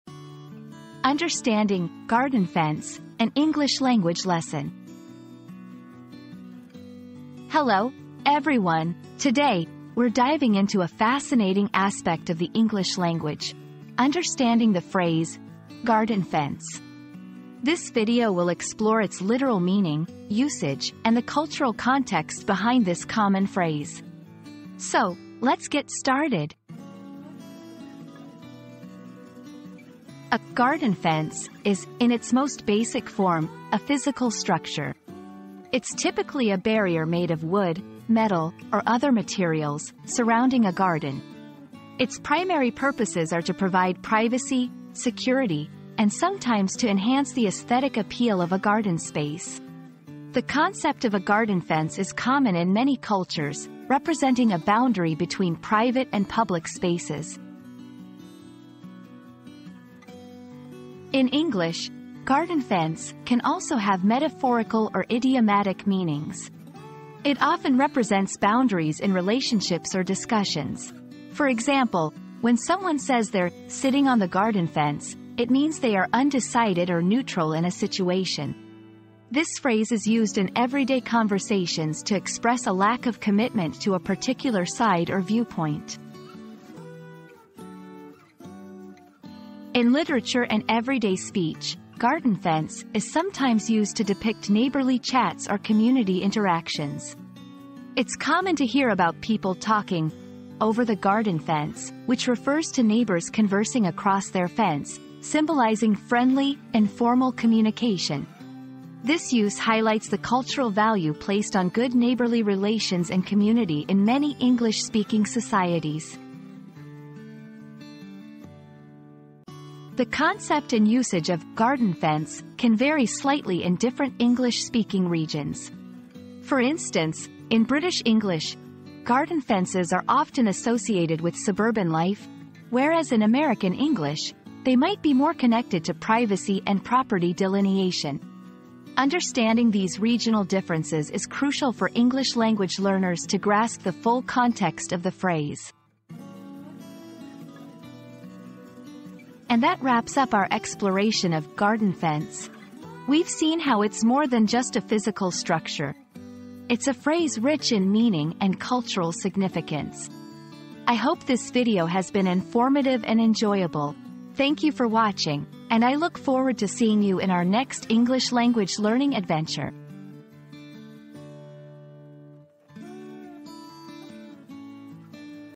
Understanding "Garden Fence": An English Language Lesson
00:00 • Introduction - Understanding "Garden Fence": An English Language Lesson
00:29 • The Literal Meaning of "Garden Fence"
01:00 • Metaphorical and Idiomatic Usage
01:29 • Cultural References and Examples
01:59 • Variations in Different English-Speaking Regions